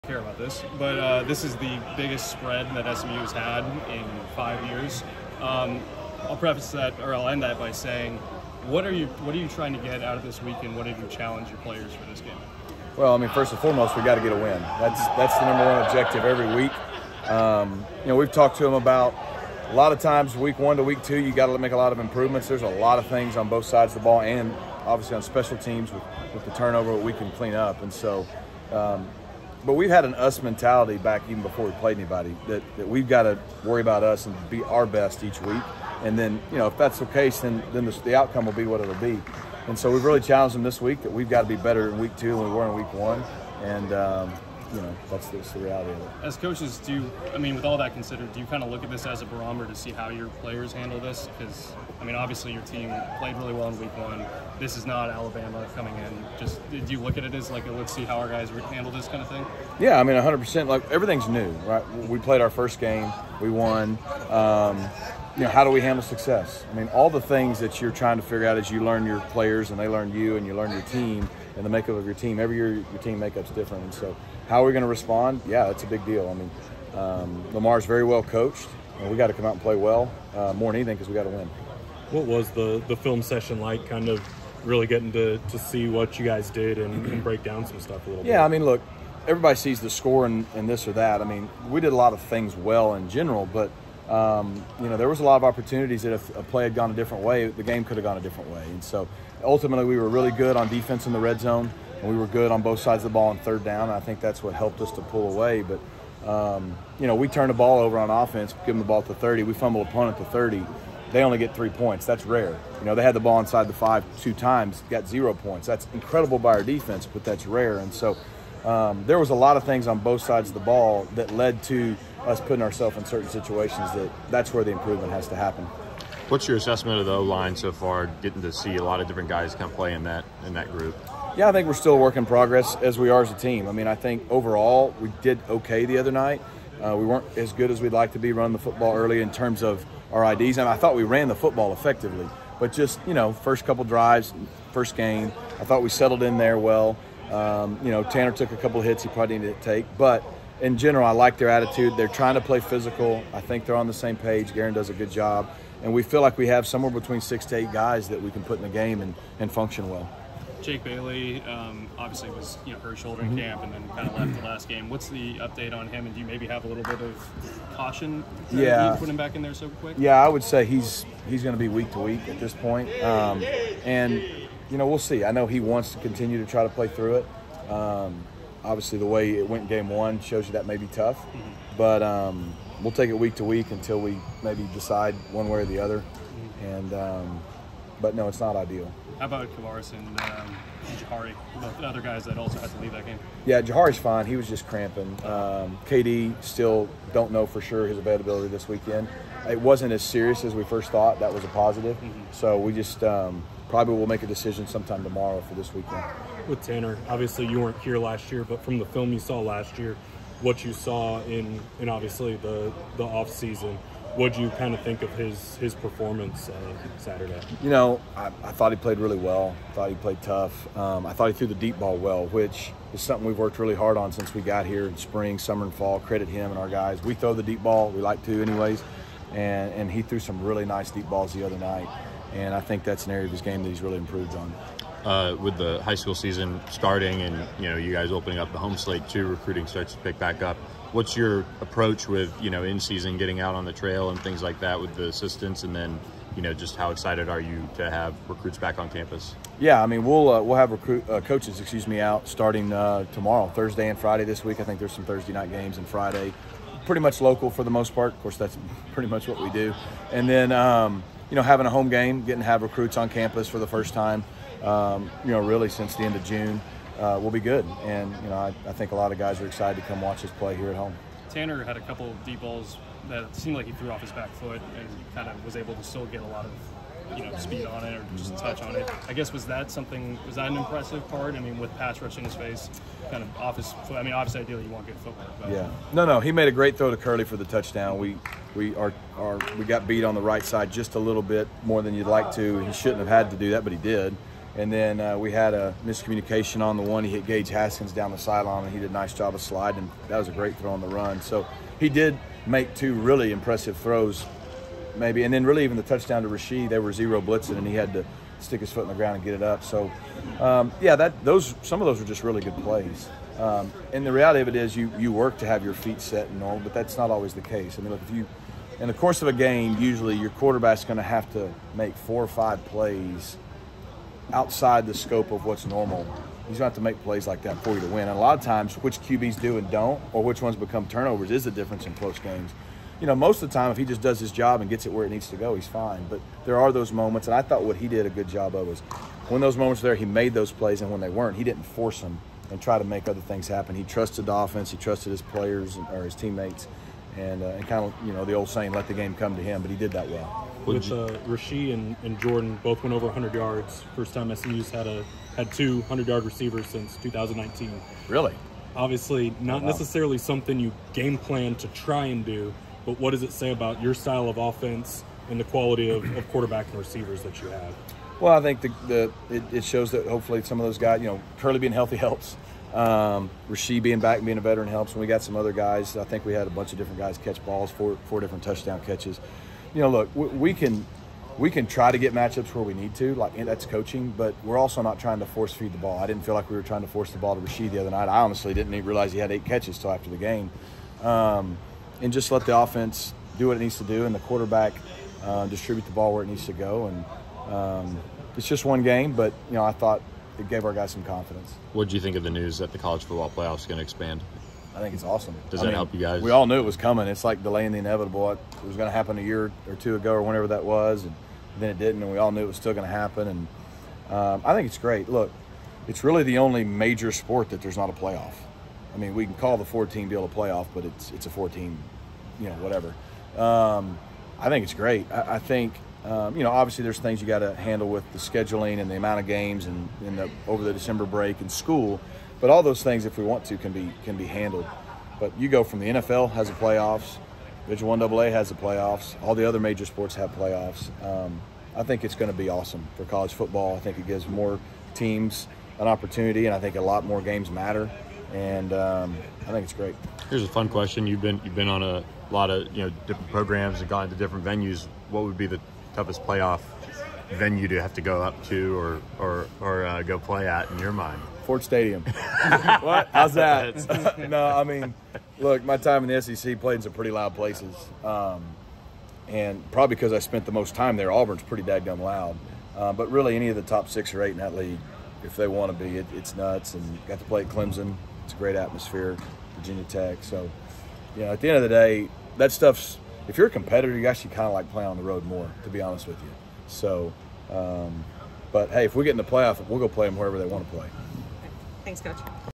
Rhett Lashlee on preparing for Lamar, injury updates and SMU offensive production vs. North Texas
Head coach Rhett Lashlee shared his thoughts on preparing for Lamar, injury updates, SMU offensive production vs. North Texas and much more.

Want more coverage of SMU Football, including the inside scoop on the team and recruiting?